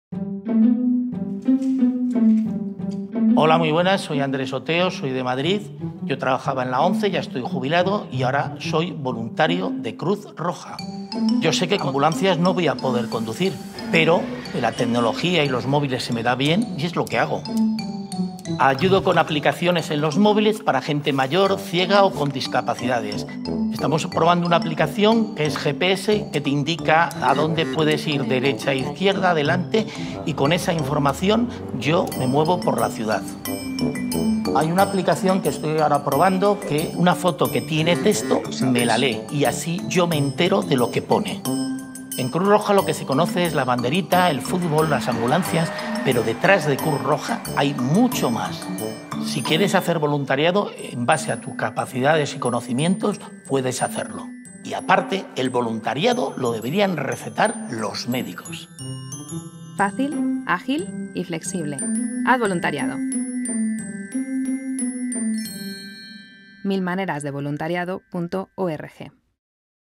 Andrés (Euskera)
"Andrés opina que todos podemos hacer algo por los demás, por eso, ayuda a desarrollar apps que mejoren el día a día de personas invidentes como él.
Y tú, ¿te animas a hacer voluntariado?"